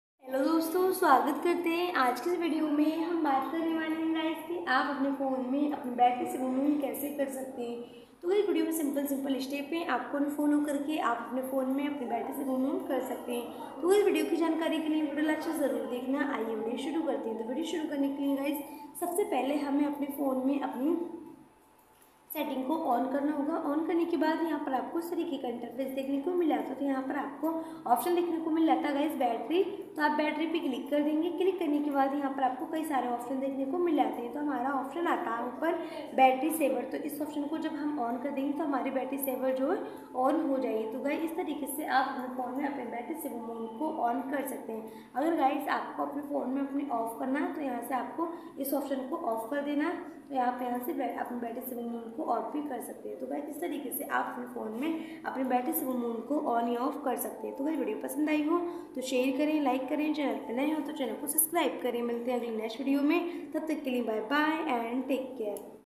How To Enable Battery Saving Mode In Redmi A1,Battery Saving Mode Kaise Enable Kare
How To Enablebattery Savingmode In Redmi A1,Battery Saving Mode Kaise Enable Kare